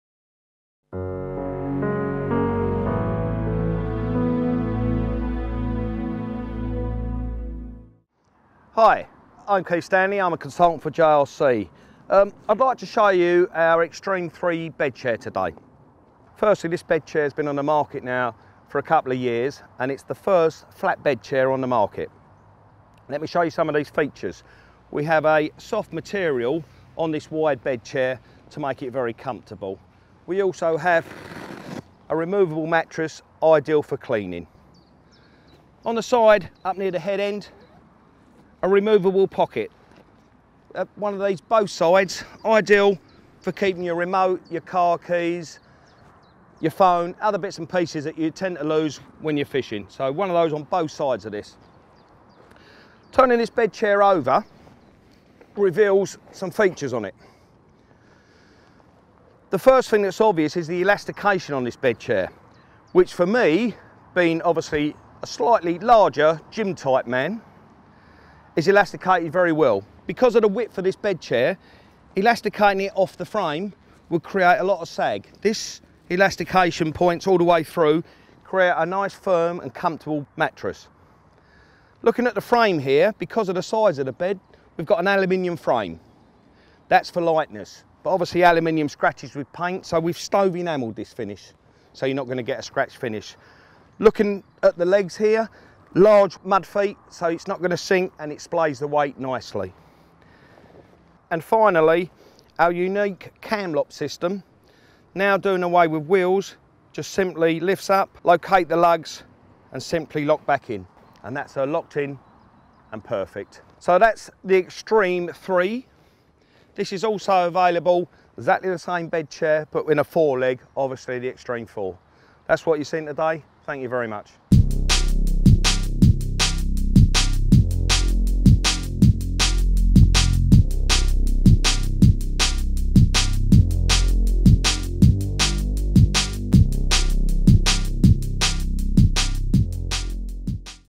JRC Extreme Three Leg Bedchair - Carp Fishing Bed Chair Product Demo
Extreme bedchairs are top of the range! Very lightweight featuring a matt coloured powder coated aluminium construction with highly efficient swivel and fold flat, extra large mud-feet. Our unique cam-locking system is an industry first, meaning that the angler does not have to fiddle with a conventional toothed cam any longer, in order to obtain the perfect adjustment and level for your bedchair. With our new cam-locking system, all you need to do is release the clasp, set to your desired position, and then simply push the cam lever back into place, and its job done! The luxuriously thick high density foam-filled mattress is fully removable and washable, and is finished off in a beautiful peach skin fabric for the extreme in bank side comfort! Find out more about the JRC Range of carp angling products